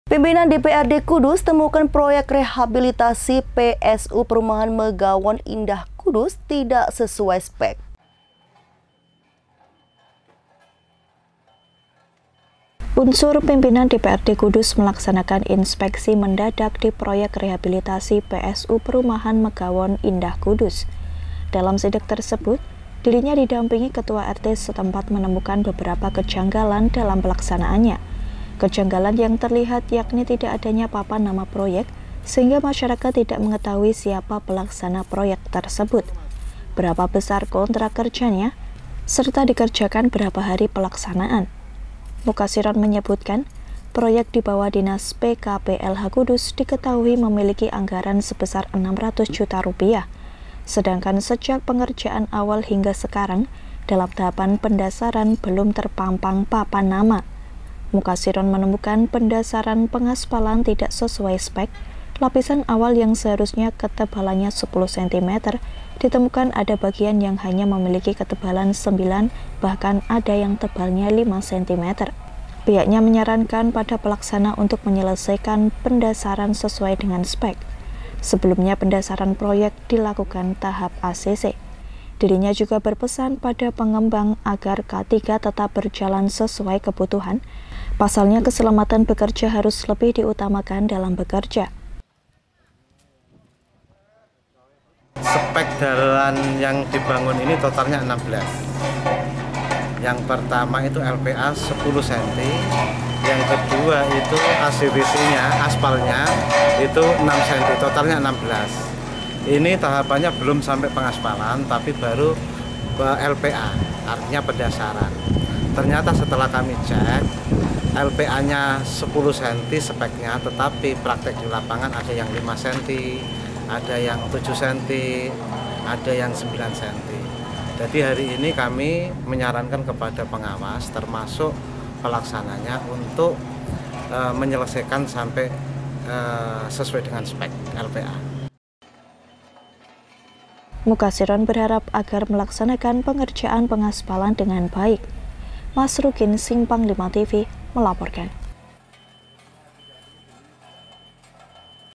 DITEMUKAN PROYEK PENGASPALAN PERUMAHAN MEGAWON TAK SESUAI SPEK
PIMPINAN DPRD KUDUS TEMUKAN PROYEK REHABILITASI PSU PERUMAHAN MEGAWON INDAH KUDUS/ TIDAK SESUAI SPEK//